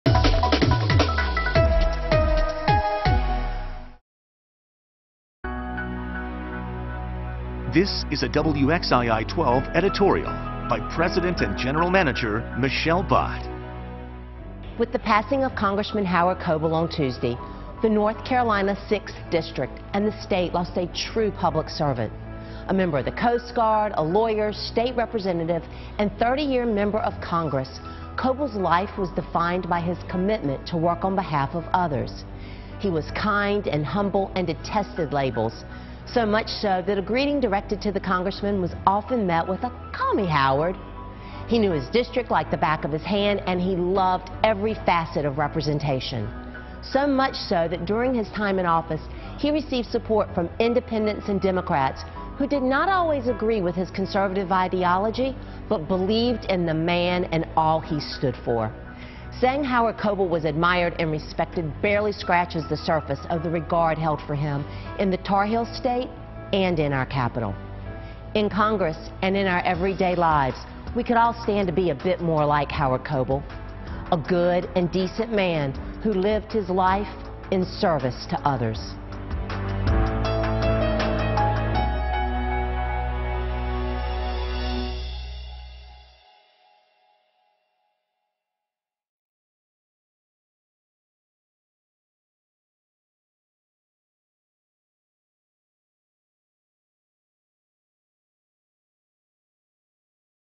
Editorial: Howard Coble's life defined by service
This is a weekly editorial by WXII President and GM Michelle Butt.